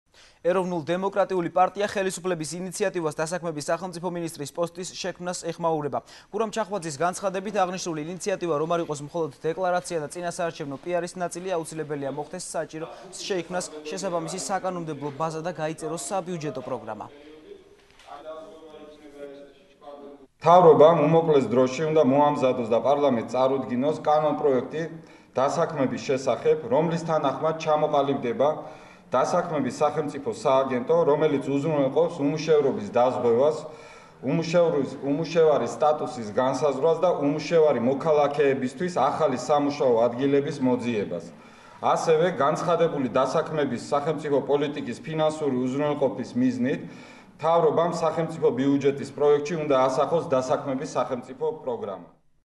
ახალი 3 | ედპ-ს განცხადება | 03.07.12
ეროვნულ დემოკრატიული პარტია ხელისუფლების ინიცატივას,  დასაქმების სახელწიფო მინისტრის პოსტის შექმას ეხმაურება.  გურამ ჩახვაძის განცხადებით აღნიშნული ინიციატივა რომ არ იყოს მხოლოდ დეკლარაცია და წინასაარჩევნო პიარის ნაწილი, აუცილებელია მოხდეს საჭიროა შეიქმნას შესაბამისი საკანონმდებლო ბაზა და გაიწეროს საბიუჯეტო პროგრამა.